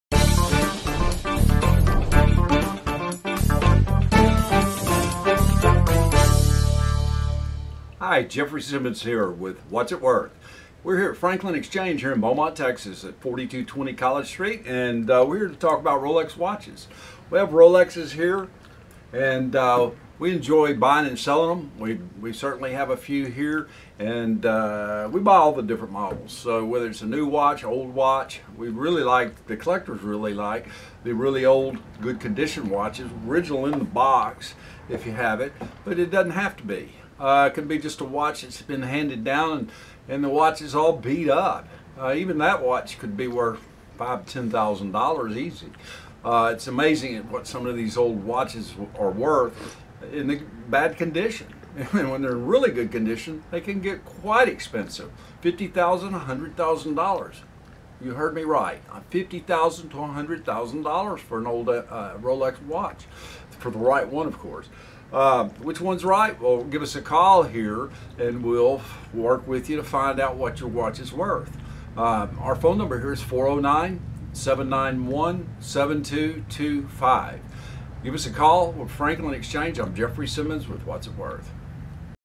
Jeffery from What's It Worth With Jeffery Simmons talking about Rolex watches.
Are you ready to sell your Rolex? Jeffery from What's It Worth With Jeffery Simmons is here to tell you more about your watch.  Check out his Facebook page What's It Worth With Jeffery Simmons and if you have any questions he will be happy to assist you.